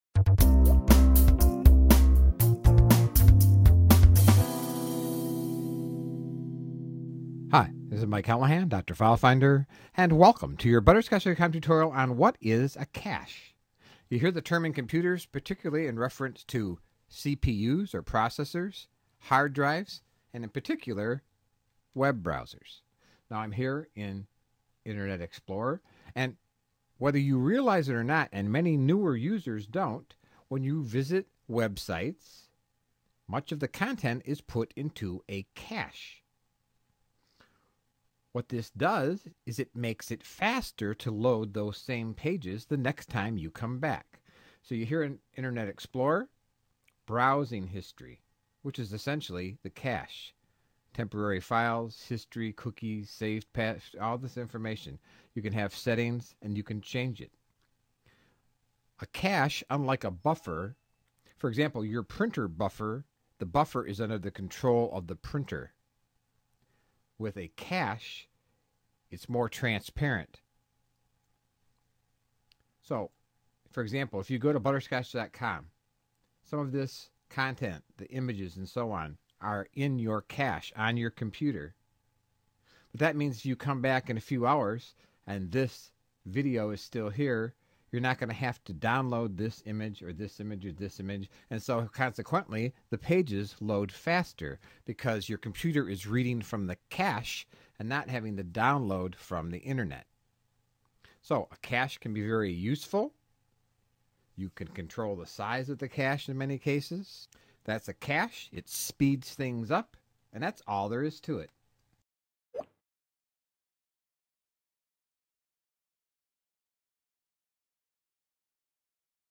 What is cache
You hear the term "cache" all over. People will ask, "How large is your cache?" or "Have you cleaned your cache out?" Well, first it helps to know what the cache is! Michael "Doctor File Finder" Callahan explain. A Internet tutorial by butterscotch.com.